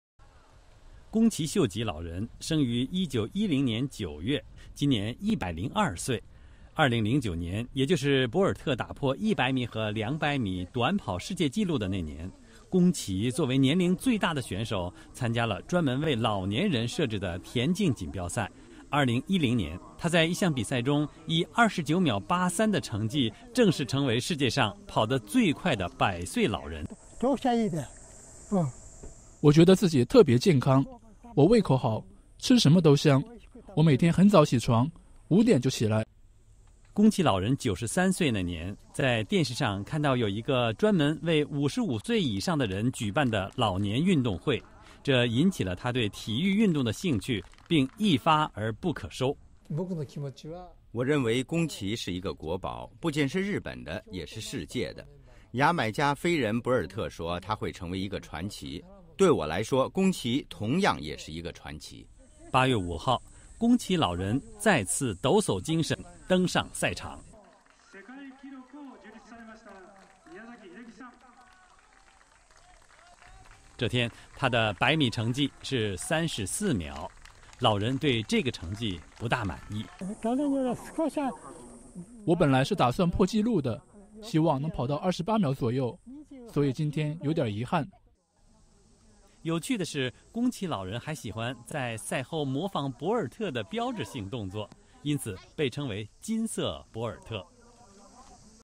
日百岁老人参加百米短跑比赛
牙买加短跑选手博尔特是当今世界第一飞人，而日本有一位被称为"金色博尔特"的百岁飞人。脸书: